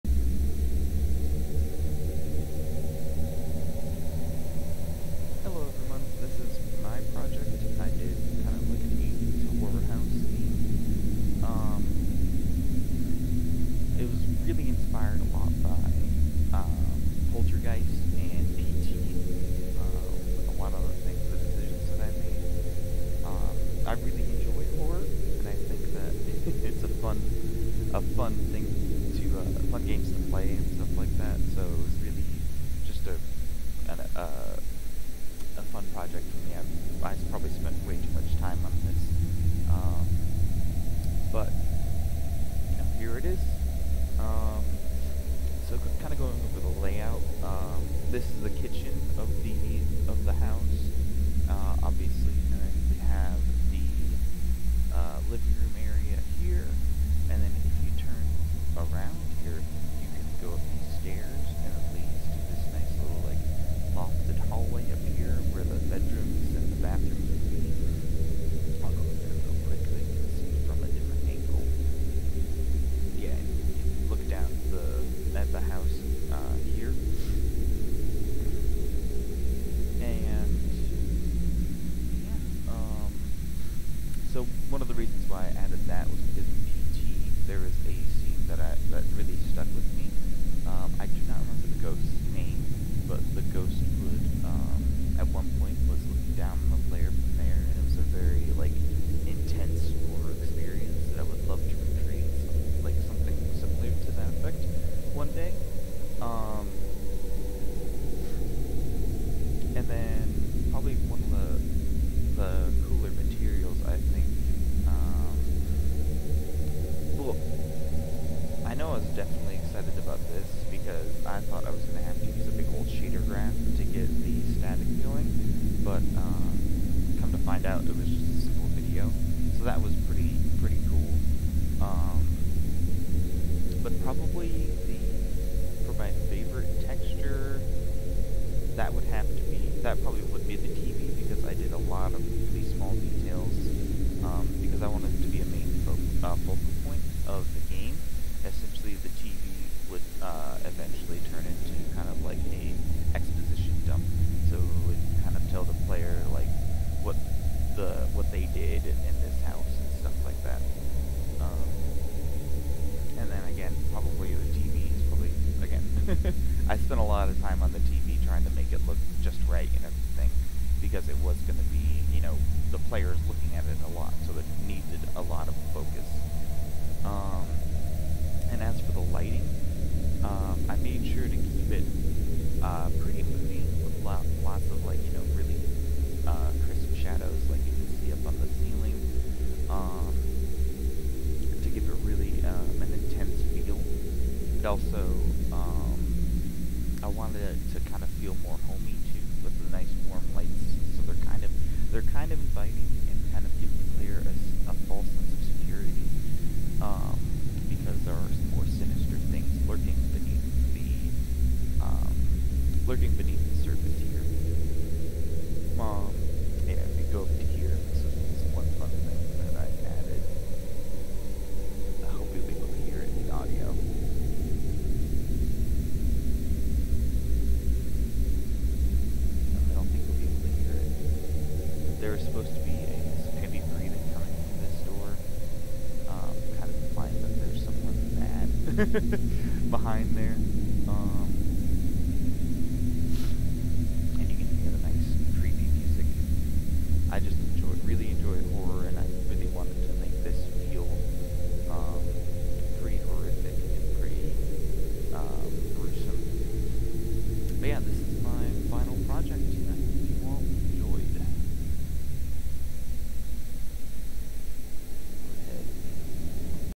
Gallagher FinalProject
This is my final 80s horror house for virtual environments